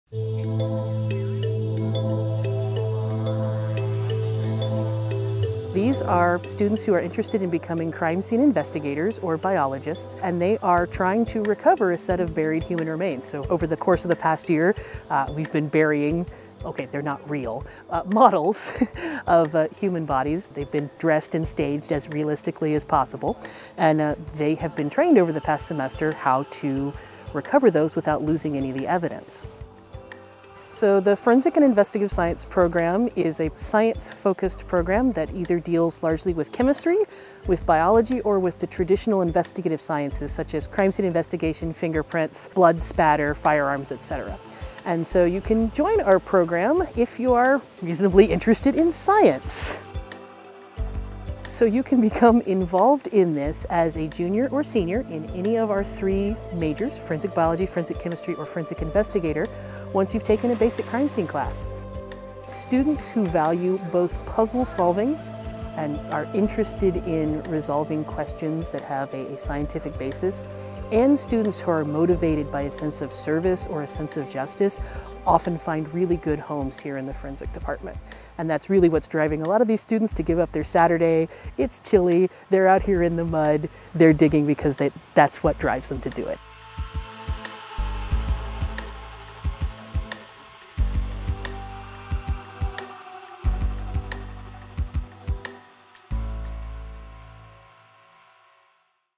REAL LIFE CSI AT WVU 🧐🔬🚨
What's it like to be a crime scene investigator? Watch as our Forensic and Investigative Science students dig up bones to recover evidence.

Learn more about our majors, scholarships, visiting, requesting information and applying